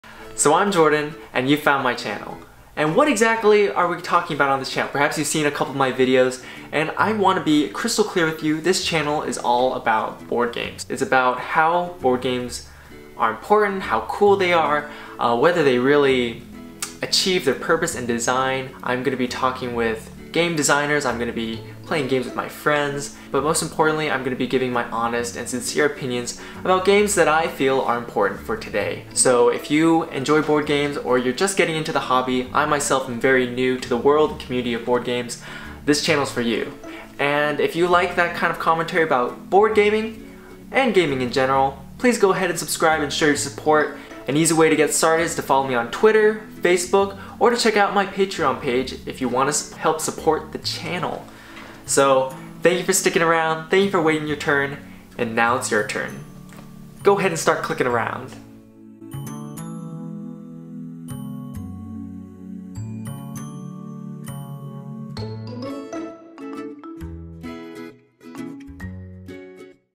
Hello! (Boardgames Weekly)
In case you were wondering, this channel is all about honest reviews, silly playthroughs, and clear critiques of board game design.

Updates Weekly